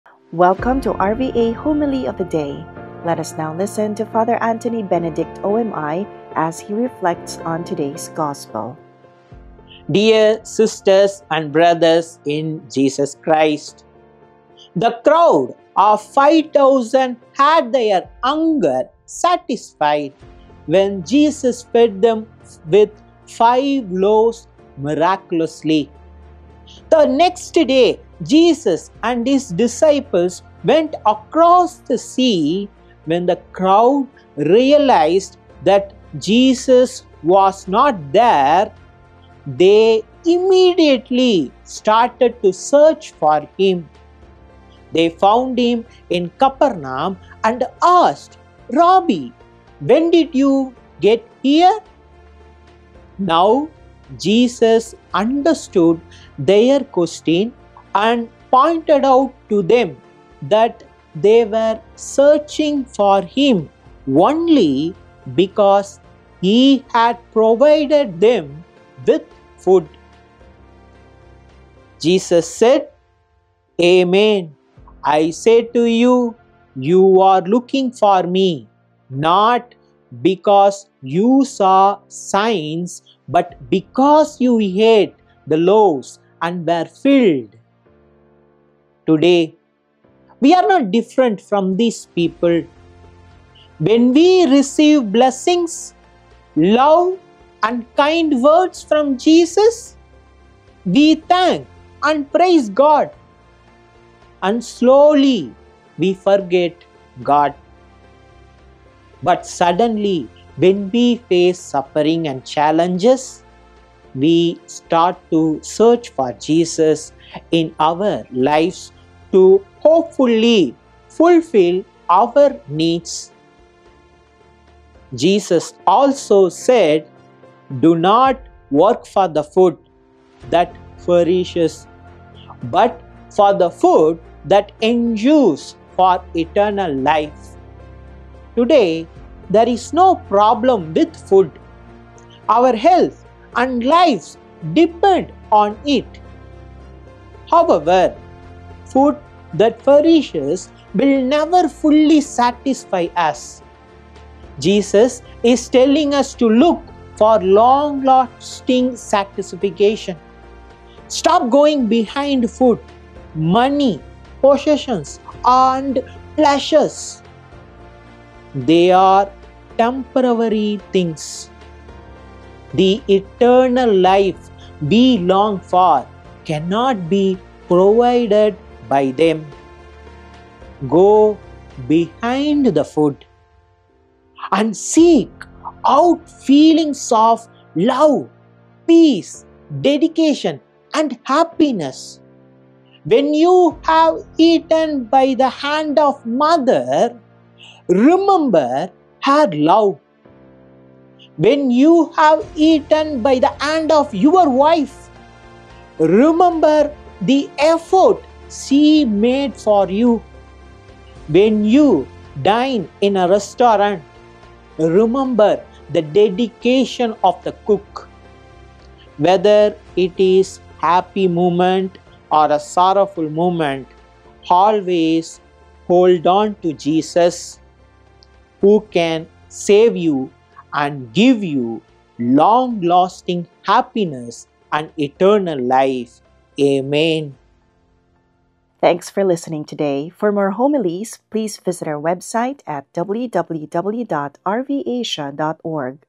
GO BEYOND THE FOOD | HOMILY OF THE DAY | April 15, 2024
HOMILY OF THE DAY - Fr. Antony Benedict, OMI

Monday of the Third Week of Easter

FULL TEXT:

GO BEYOND THE FOOD
Dear sisters and brothers in Jesus Christ
The crowd of five thousand had their hunger satisfied when Jesus fed them with five loaves miraculously. The next day, Jesus and his disciples went across the sea. When the crowd realized that Jesus was not there, they immediately started to search for him. They found him in Capernaum and asked, ‘Rabbi, when did you get here?'.

Now Jesus understood their question and pointed out to them that they were searching for Him only because he had provided them with food. Jesus said, "Amen, I say to you, you are looking for me not because you saw signs but because you ate the loaves and were filled."

Today, we are not different from these people. When we receive blessings, love, and kind words from Jesus, we thank and praise God. And slowly, we forget God. But suddenly, when we face suffering and challenges, we start to search for Jesus in our lives to hopefully fulfil our needs.

 Jesus also said, “Do not work for the food that perishes but for the food that endures for eternal life.” Today, there is no problem with food; our health and lives depend on it. However, food that perishes will never fully satisfy us. Jesus is telling us to look for long-lasting satisfaction. Stop going beyond food, money, possessions, and pleasures.

They are temporary things. The eternal life we long for cannot be provided by them. Go beyond the food, and seek out feelings of love, peace, dedication, and happiness; when you have eaten by the hand of the mother, remember her love; when you have eaten by the hand of your wife, remember the effort she made for you; when you dine in a restaurant, remember the dedication of the cook. Whether it is a happy moment or a sorrowful moment, always hold on to Jesus, who can save you and give you long-lasting happiness and eternal life. Amen